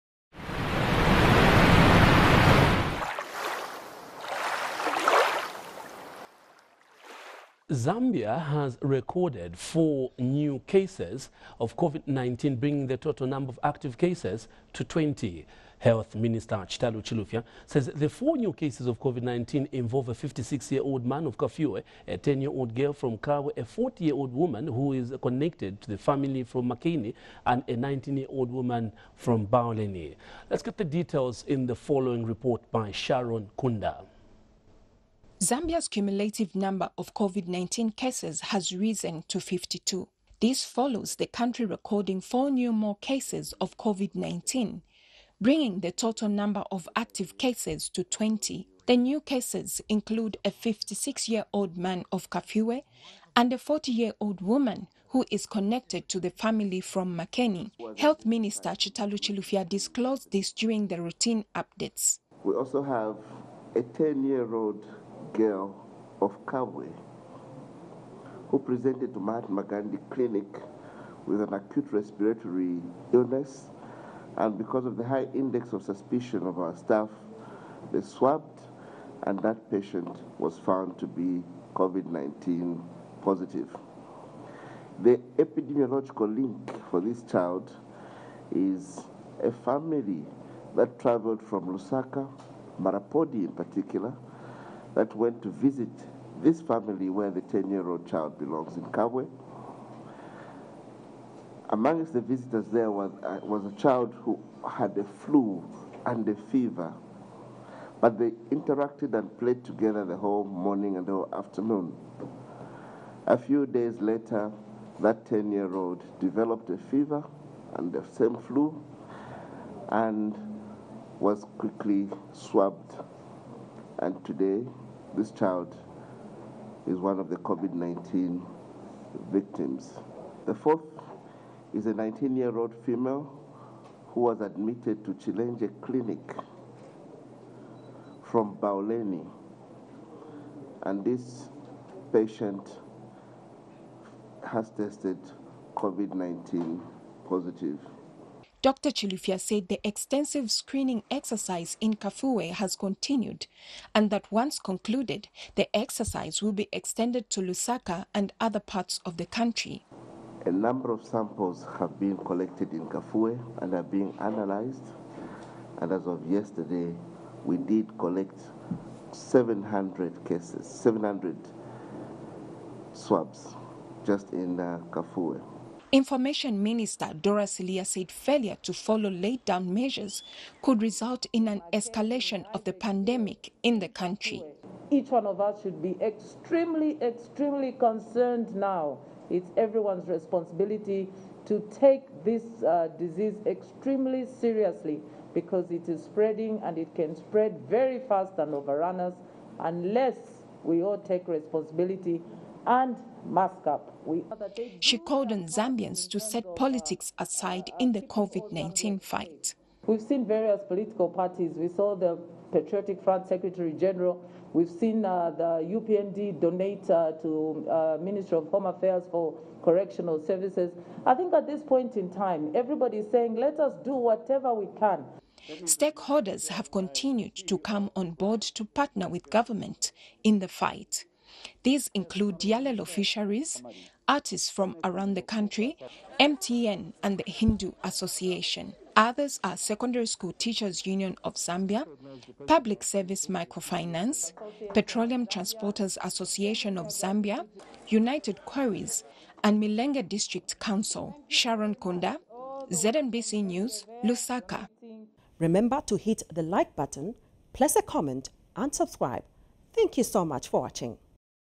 FOUR NEW COVID-19 CASES
Zambia has recorded FOUR new cases of COVID-19, bringing the total number of active cases to 20.

Health Minister CHITALU CHILUFYA says the 4 new cases of COVID-19 involve a 56 year old man of Kafue, a 10 year old girl from Kabwe, a 40 year old woman, who is connected to the family from Makeni and a 19 year old woman from Bauleni.

Speaking during the routine updates on COVID-19, Dr. CHILUFYA said the 10 year old girl from Kabwe had gone to the clinic with an acute respiratory illness and has epidemiological link to a family that travelled from Lusaka to visit in Kabwe.